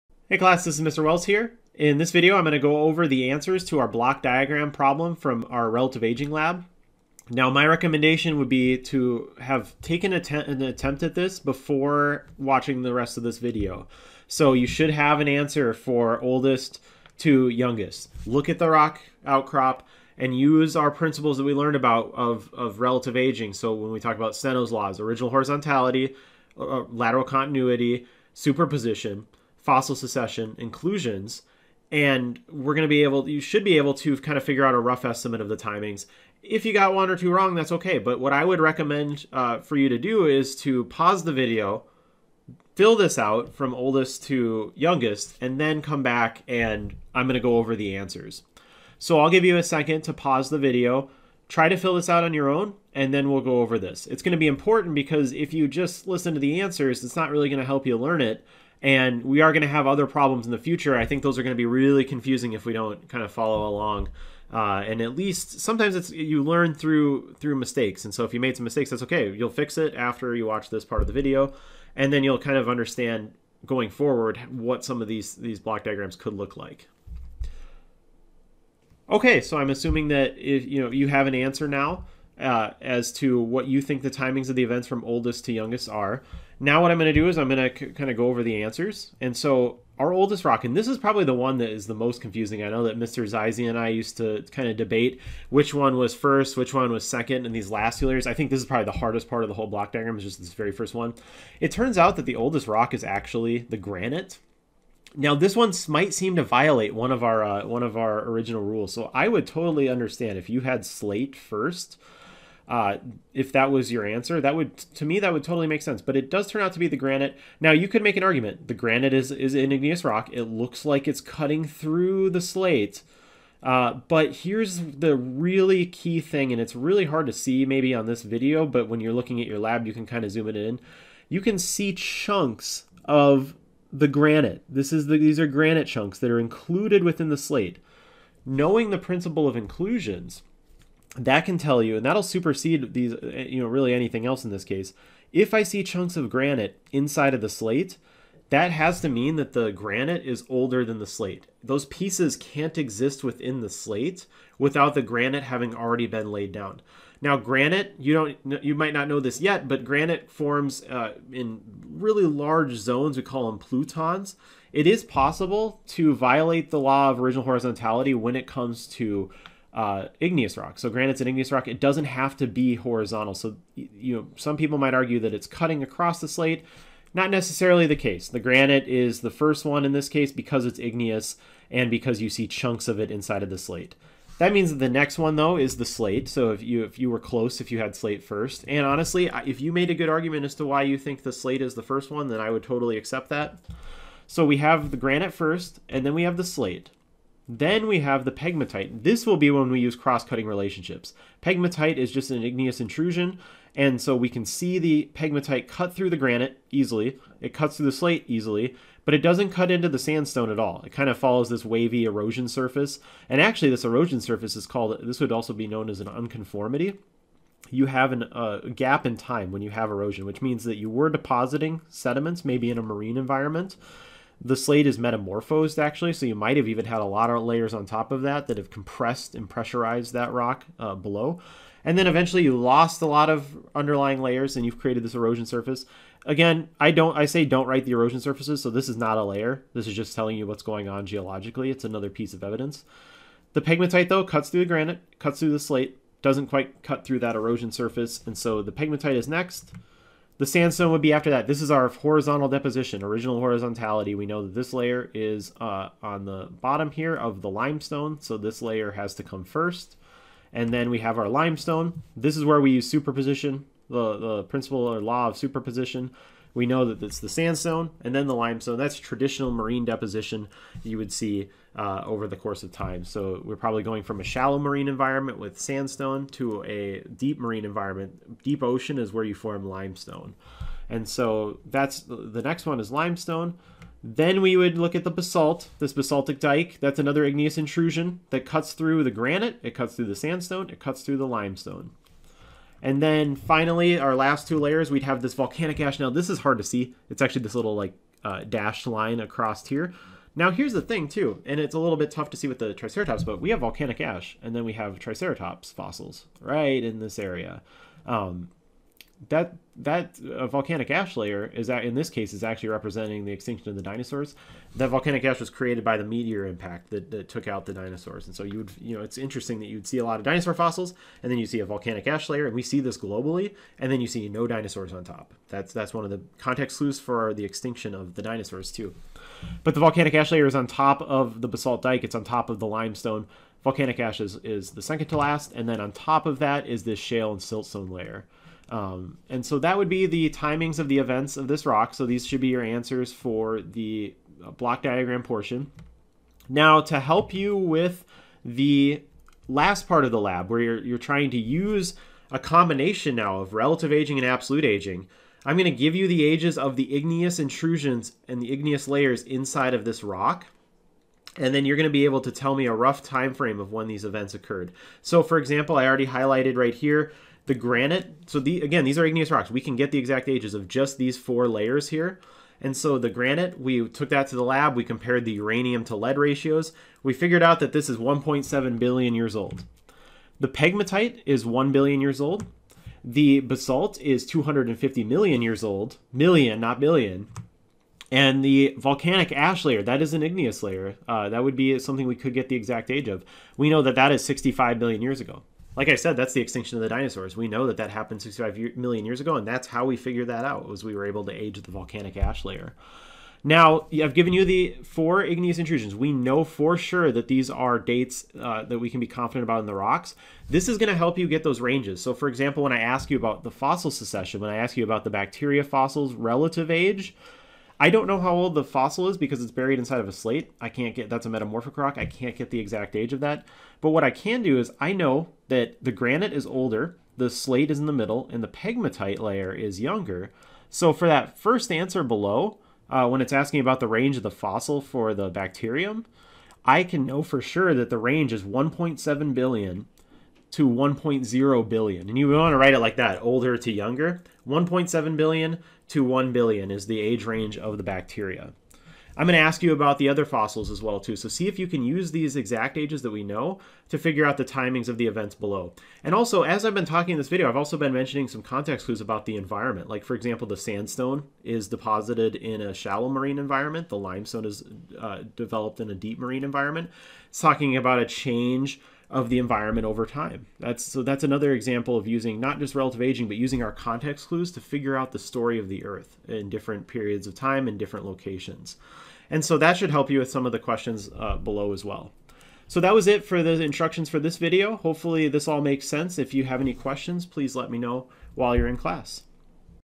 Relative Aging of Rocks Lab Block Diagram Solutions
In this short video, I will go over the solution for the block diagram portion of our Relative Aging of Rocks Lab. This problem discusses many of the key laws and principles of relative aging.